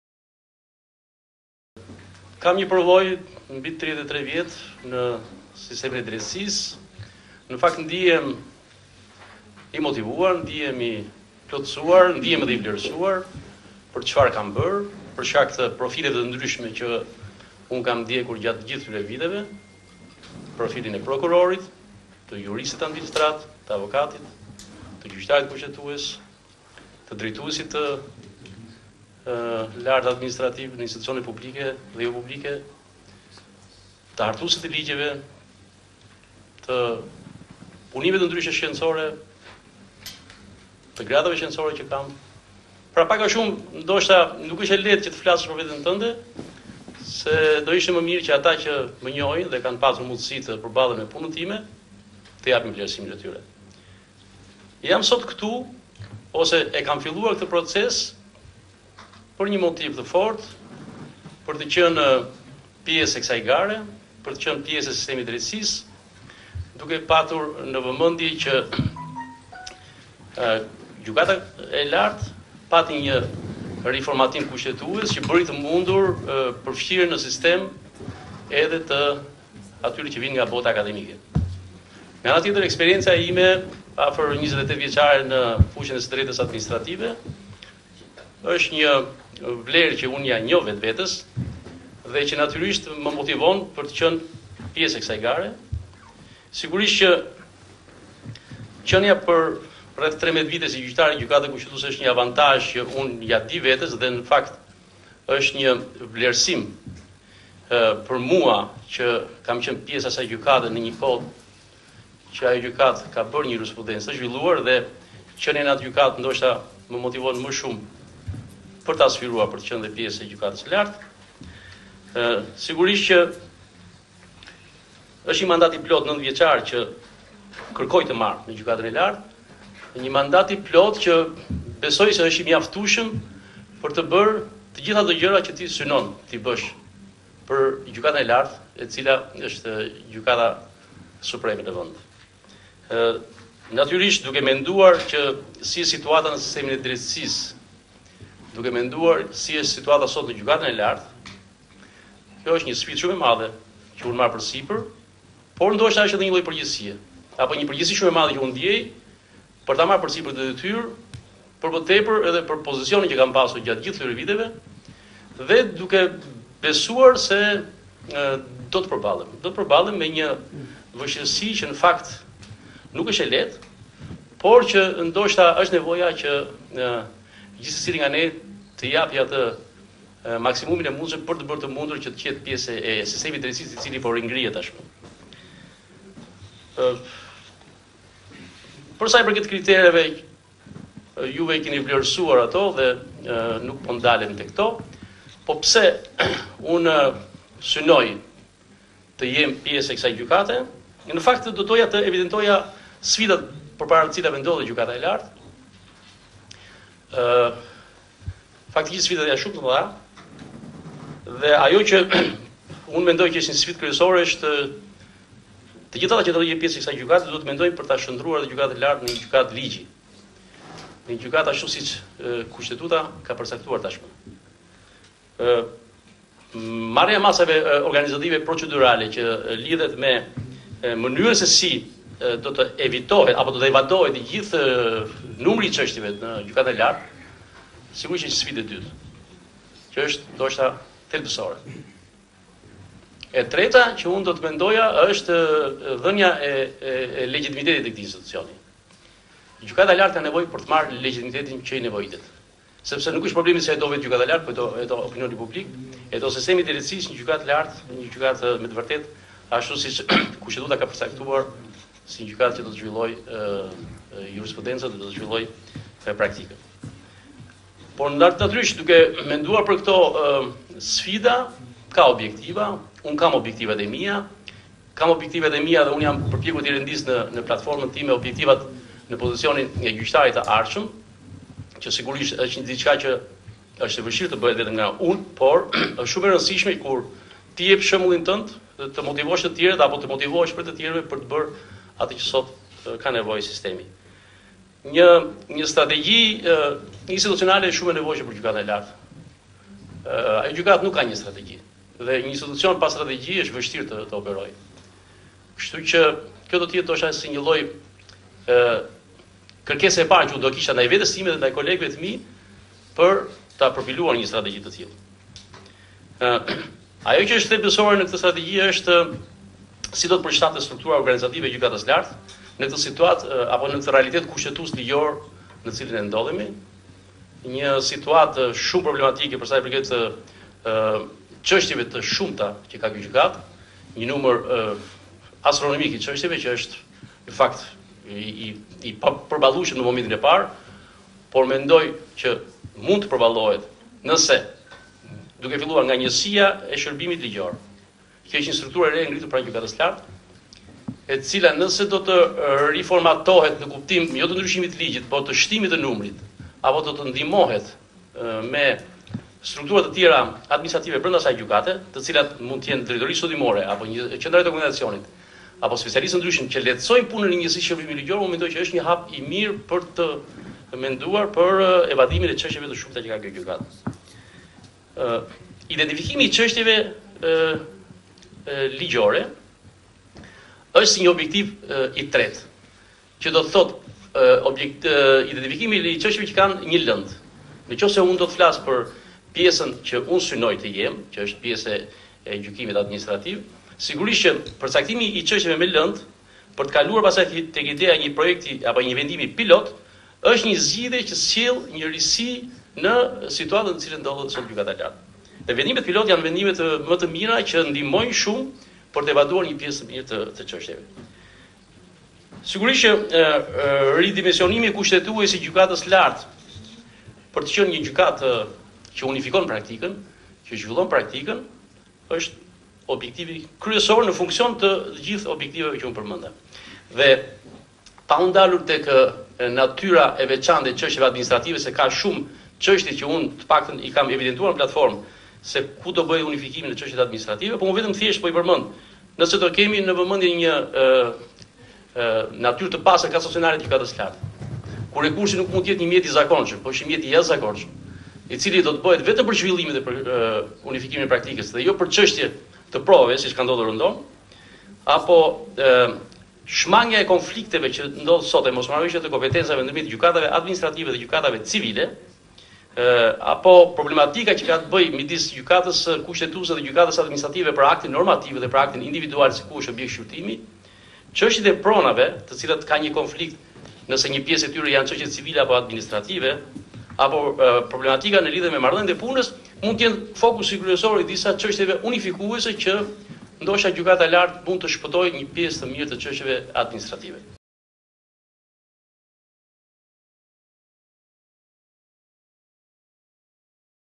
Sokol Sadushi del para KLGJ: Në Gjykatën e Lartë do jap eksperiencën time
TIRANË- Sokol Sadushi, kandidat për anëtar në Gjykatën e Lartë është intervistuar nga Këshilli i Lartë Gjyqësor. Para anëtarëve të KLGJ, Sadushi tha se  ai ka një përvojë 33-vjeçare dhe shtoi se të gjithë ekperiencën e tij do e japë nëse do jetë anëtar në Gjykatën e Lartë. Më tej tha se e kërkon këtë detyrë se donte të përballej dhe ishte i motivuar dhe bashkë me kolegët ta kthente Gjykatën e Lartë ashtu siç e do sistemi i drejtësisë.

“Kam një përvojë mbi 33 vjeçare si jurist, avokat, prokuror e më gjerë dhe jam sot këtu për një motiv të fortë, për të qenë pjesë e garës që Gjykata e Lartë pati riformatim, Gjykata e Lartë ka nevojë për legjitimitetin që i duhet, siç kushtetuta e ka përcaktuar. Kam objektivat e mia në lidhje me këtë gjykatë. Aktualisht kjo gjykatë nuk ka strategji dhe kjo pikërisht i duhet. Situatë problematike janë numri i stërmadh i dosjeve në këtë gjykatë, por mund të përballohet me një riformatim. Çështja e pronave dhe disa të tjera mund të bëhen unifikuese, për të shpëtuara nga ata.
Në përvojën time ekzistojnë rregullore të tilla për të lehtësuar punën. Me rëndësi është roli i gjykatës me median, nuk ka qenë në rregull, por duhet të përmirësohet që publiku të informohet me transparencë. E kërkoj këtë detyrë se dua të përballem, jam i motivuar dhe bashkë me kolegë të kthejmë Gjykatën e Lartë ashtu siç e do sistemi i drejtësisë. Pa unifikimin e praktikës gjyqësore nuk mund të kemi evadim të çështjeve, pasi duhen vendime unifikuese nëse duam të gjykojmë në kohë çështjet. Sa më shpejt, aq më shpejt do marrë zgjidhje dhe qytetari që pret prej vitesh një vendim”, ishte kjo një pjesë nga deklarata e Sadushit.